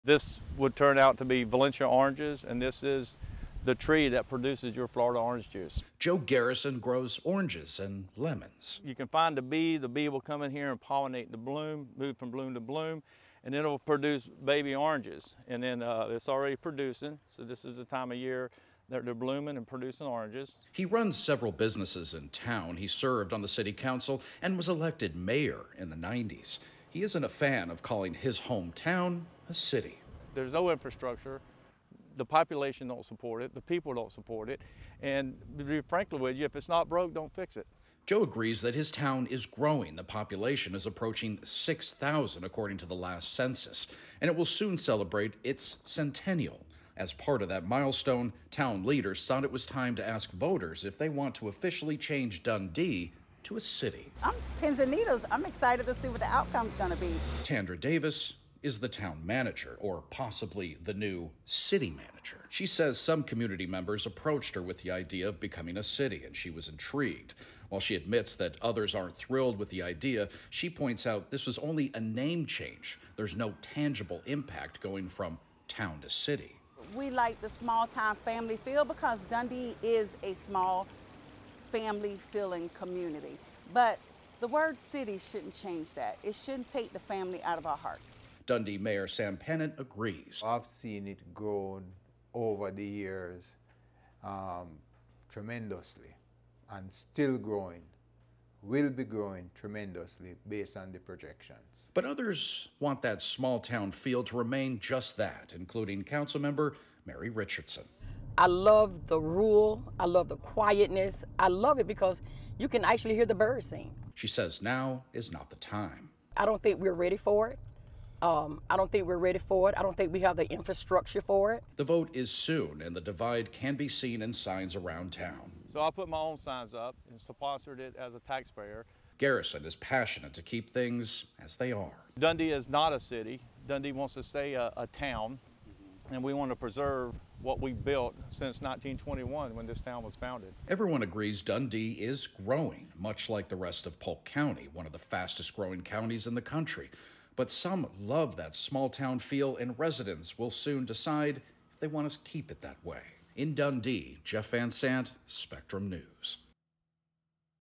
Dundee, Florida might become a city.
The small community of Dundee will vote to decide if it wants to become a City.